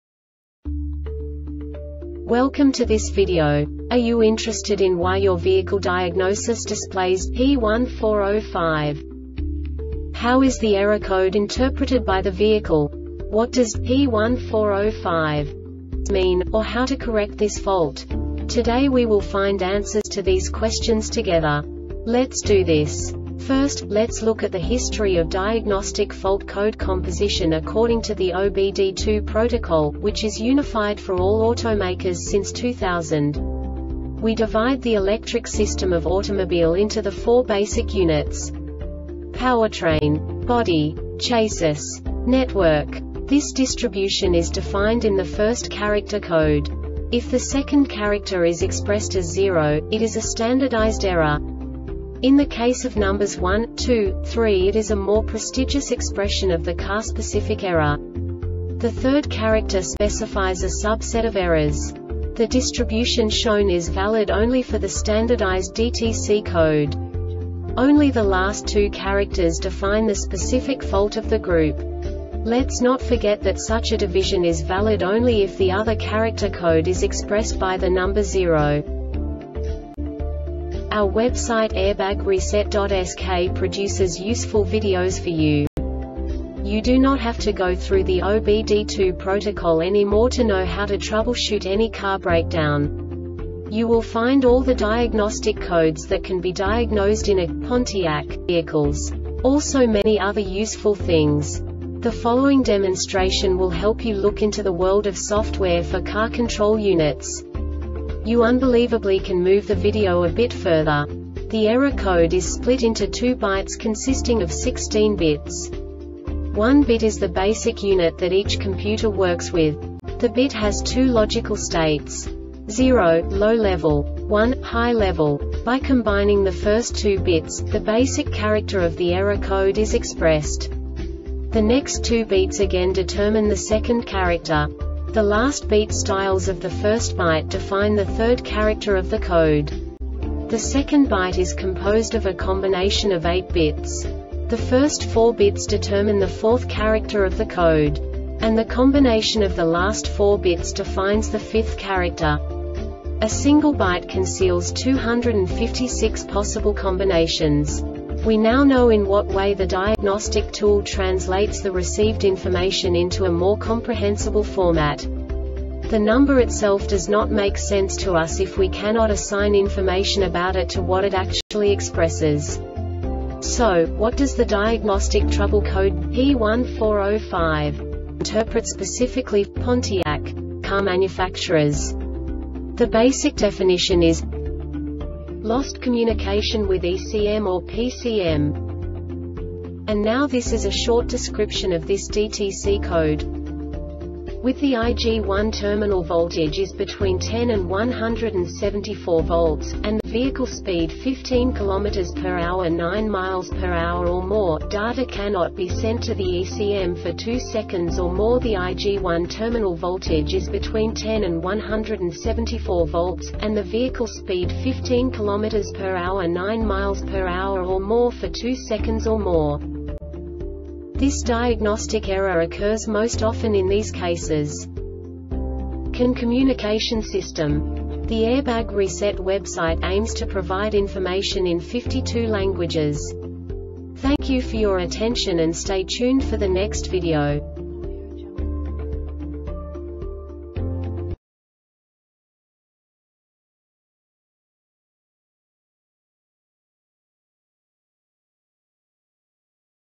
DTC Pontiac P1405 Short Explanation
Contents:
0:21 Basic DTC analysis according to OBD2 protocol standard.
1:48 Insight into programming
2:58 A diagnostically understandable description of the Diagnostic Trouble Code
3:05 Basic error definition

Make:
Pontiac
Code:
P1405
Definition:
Lost Communication with ECMorPCM
Description:
With the IG1 terminal voltage is between 10 and 17.4 V,and the vehicle speed 15 km or h (9 mph) or more,data cannot be sent to the ECM for 2 seconds or more.The IG1 terminal voltage is between 10 and 17.4 V,and the vehicle speed 15 km or h (9 mph) or more for 2 seconds or more.